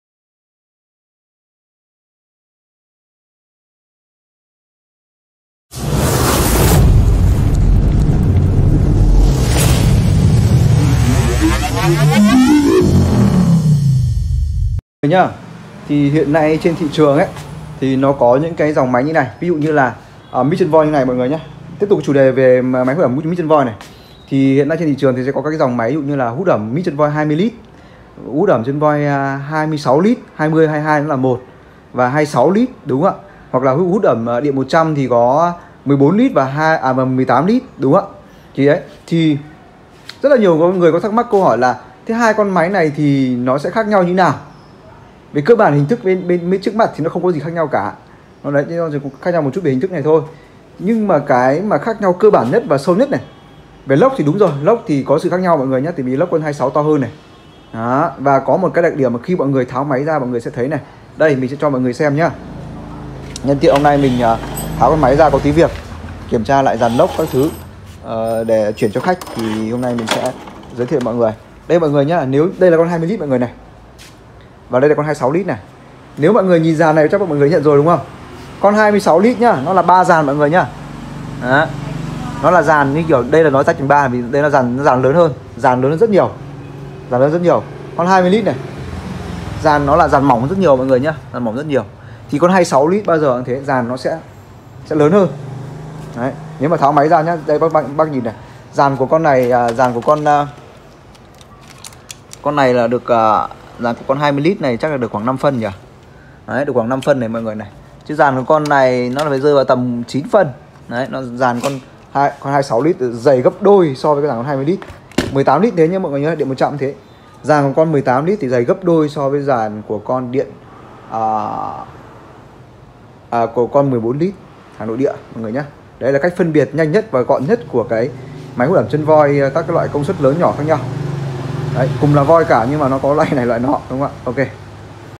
Phân biệt máy hút ẩm Mitsubishi Nhật Chân voi loại 20 lít và 26 lít điện 220v - Điện Máy An Gia
Điện máy An Gia sẽ phân tích điểm khác nhau giữa máy hút ẩm Mitsubishi chân voi hàng nhật công suất 14 lít điện 100v, 20 22 lít điện 220v với 18 lít điện 100v và 26 lít điện 220v. Điểm khác nhau dễ nhận thấy nhất. Mọi người cùng theo dõi